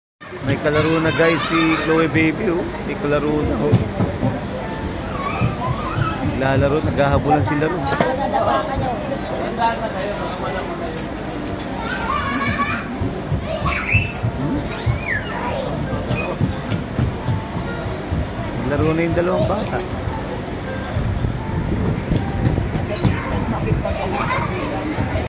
BYAHENG HALIMAW SA KOPA KOPPI HANGGANG SA CAFE AGAPITA DITO LAMANG SA TAGAYTAY CITY Episode 42
BYAHENG HALIMAW SA KOPA KOPPI HANGGANG SA CAFE AGAPITA DITO LAMANG SA TAGAYTAY CITY

Episode 42